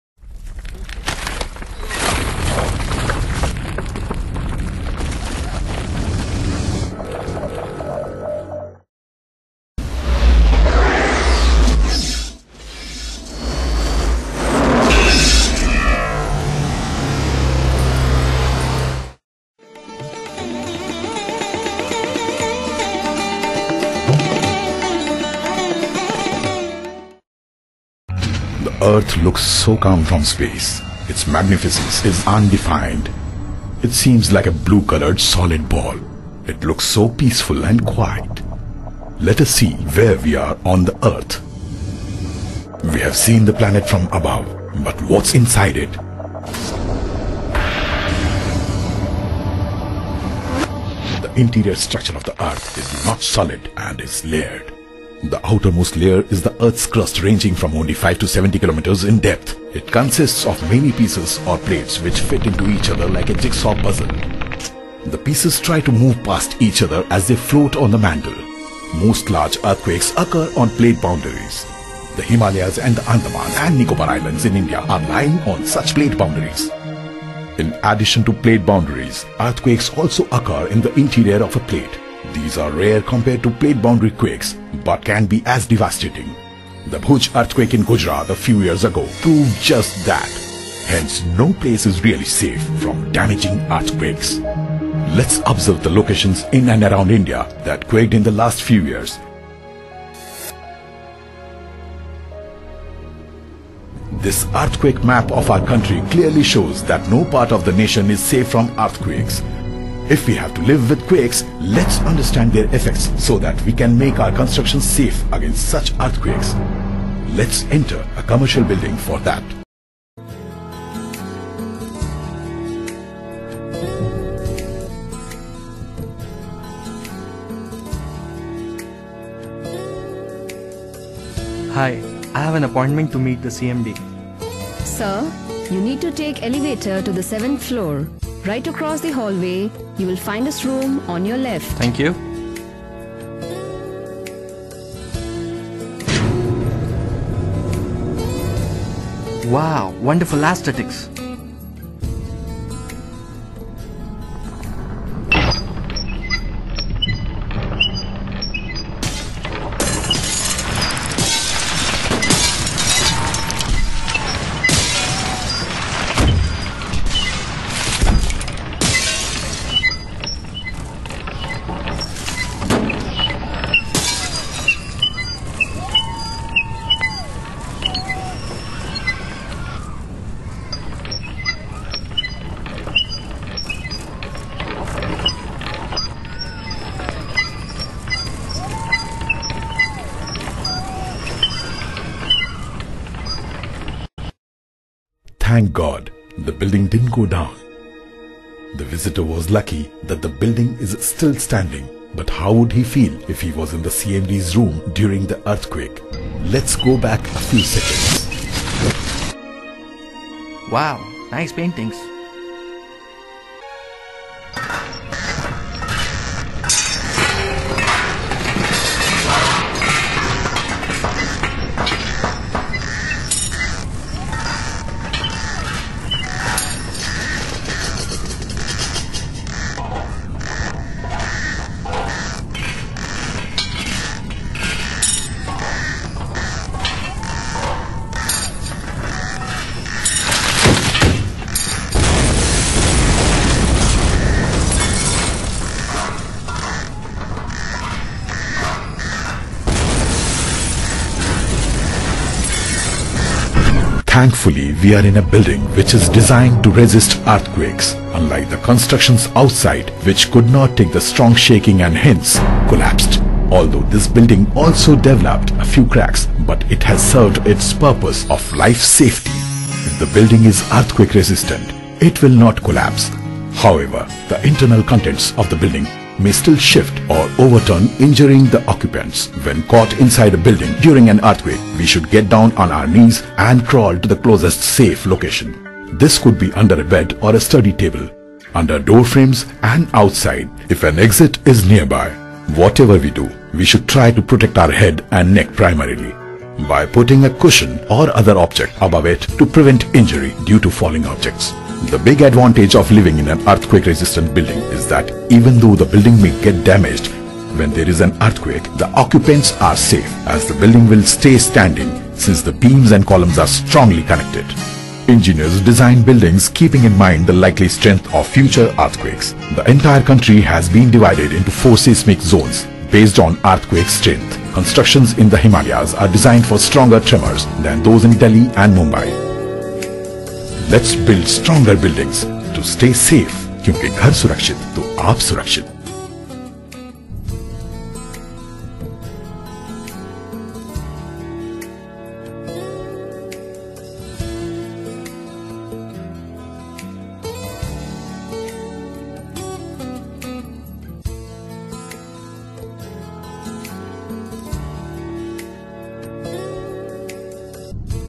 Earthquake Simulation
Buiding damage during earthquake and safety tips